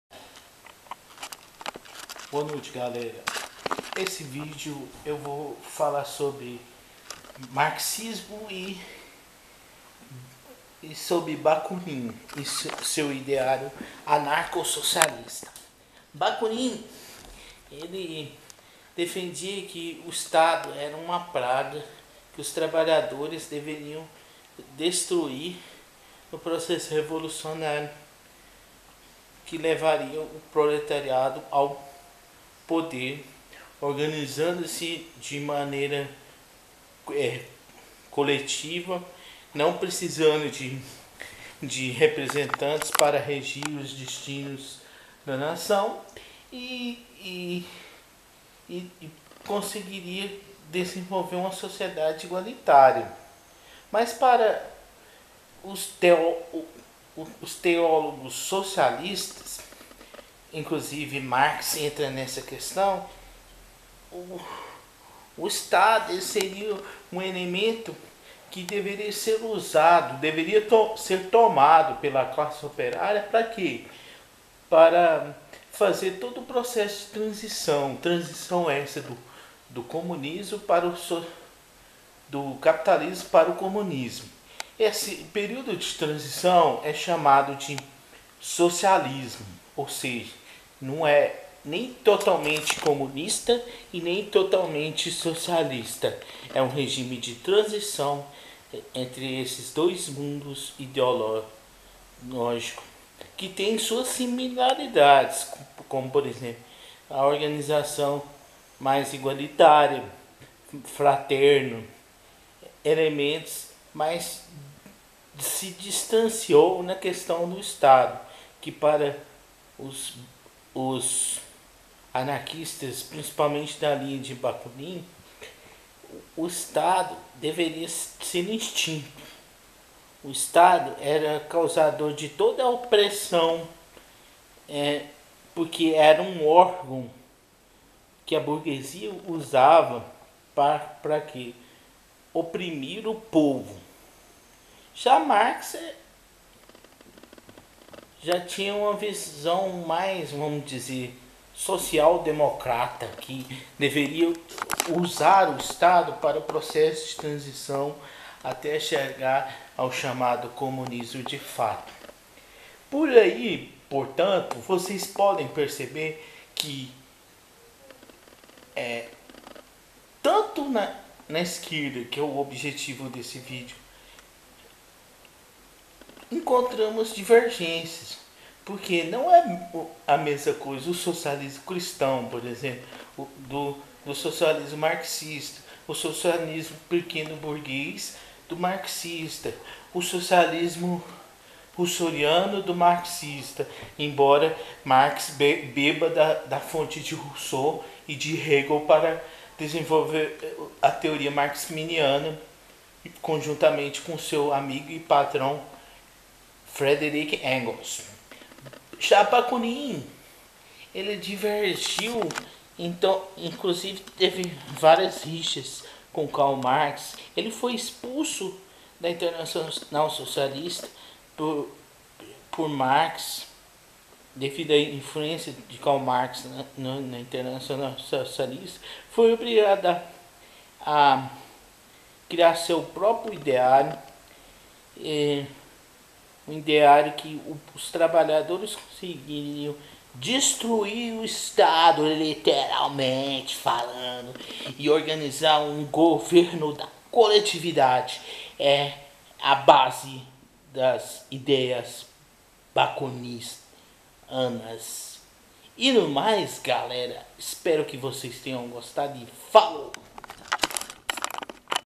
Qual é a diferença entre socialismo e anarco-socialismo ?
Este vídeo que irei produzir tem como proposito diferenciar o Socialismo Maximiano e o Anarco socialismo de Mikhail Bakhtin que tem suas similaridades, contudo se rivaliza com relação a concepção de Estado.
Para os Marxistas, o Estado é o elemento que tem como função de fazer a ponta da transição da sociedade Capitalista ate que se desenvolve  se o chamado Comunismo e o mesmo Estado seria comandado por trabalhadores proletários aos quais Marx e Engels denominou de ditadura do proletário.
Já o chamado Anarco socialismo cujo um dos maiores expoentes do Ideário em questão foi Mikhail Bakhtin rival de Marx que defendia a revolução como forma de mudança para chegar a uma sociedade igualitária, todavia não defendia a ditadura proletária de Marx porque tinham como concepção que o Estado é um dos  agentes  da opressão portanto defendiam que o Estado deveria acabar. É Importante dizer que não existe somente uma forma de anarquismos, pois além do Anarco socialismo também existe um anarco-capitalismo que é uma ideia da extrema direita global.  Portanto podemos concluir que para uma pessoa criticar o socialismo deve saber que socialismo ele quer criticar como os dois exemplo colocado apesar de ter similaridades porque defendia o social como principio as duas ideologias se rivalizava no tocante a forma da revolução sendo que para o socialismo de Marx o Estado é um agente de transformação que deveria ser controlado pela classe proletária ate que se chegue a sociedade perfeita onde a fraternidade seria o principio basilar e a igualdade de oportunidade seria uma pratica comum. Todavia o Anarco socialismo defende que o Estado deveria acabar e o governo deveria ser regido por entidades trabalhistas, ou seja, pelos trabalhadores, tendo similaridades neste ponto com o Anarco capitalismo só diferenciado na questão de quem substituiria o Estado que para o capitalismo que tem ligações com as igrejas diferente do anarco socialismo que defendia que as religiões também era um braço da opressão.

Pesquisas